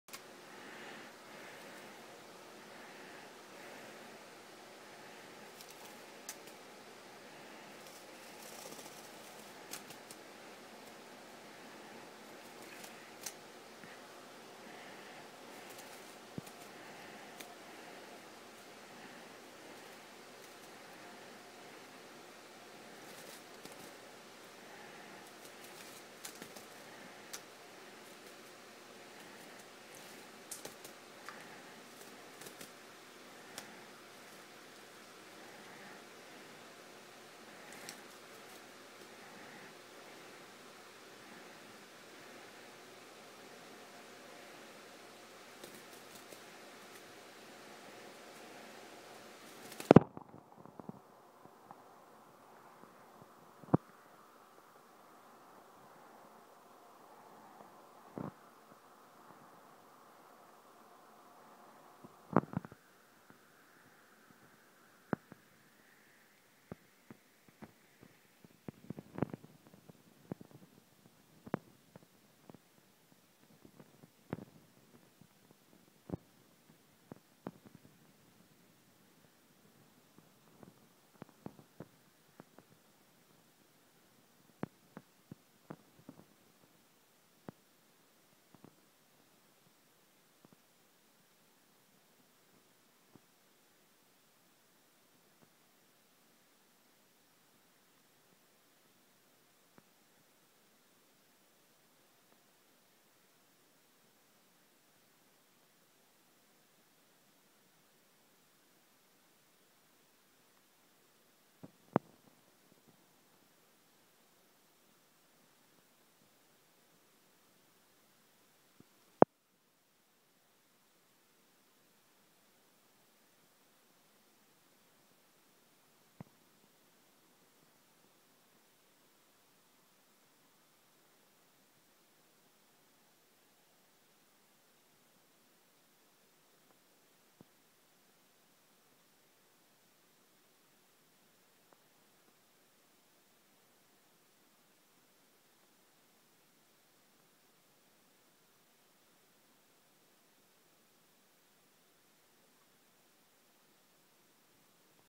My GC Conure "Chirpy" is bathing
My Yellowsided Green Cheek loves nothing more than washing herself in fresh drinking water.